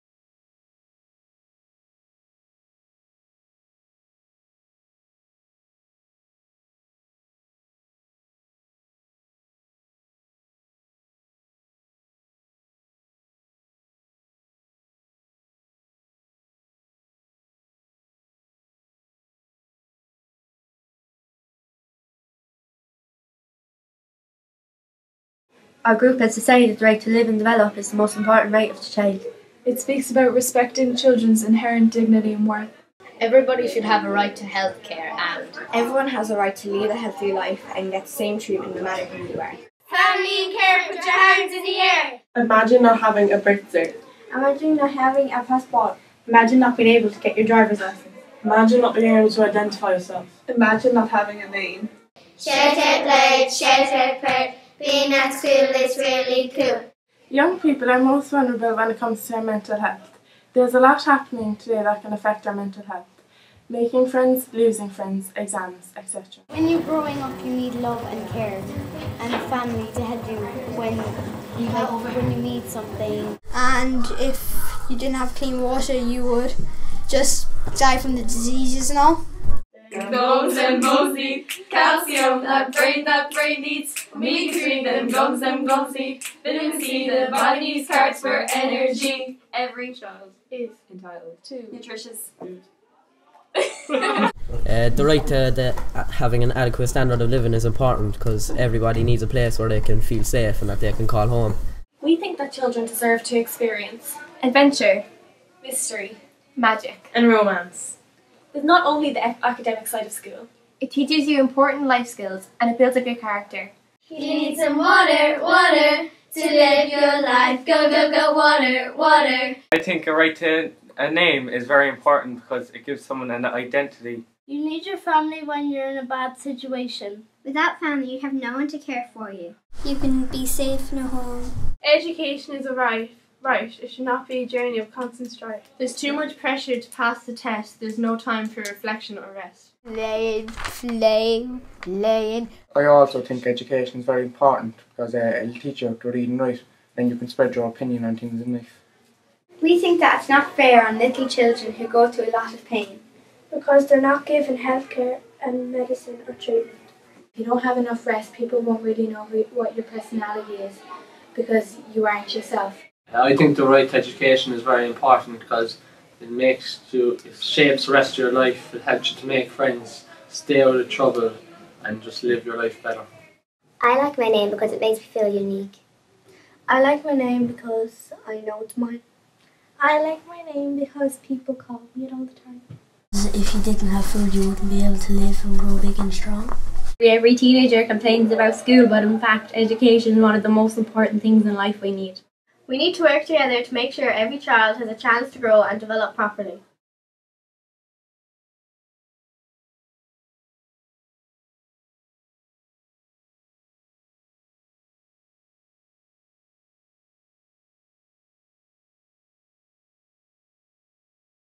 Children's right to survival and development
Children's right to survival and development under the UN Convention on the Rights of the Child include rights to adequate nutrition, clean water, housing, healthcare, education, a name and nationality, family and care, rest and play.

In this clip  - shot over 4 month period in 2013 in the Ombudsman for Children's Office, children and young people share their thoughts on some of these rights.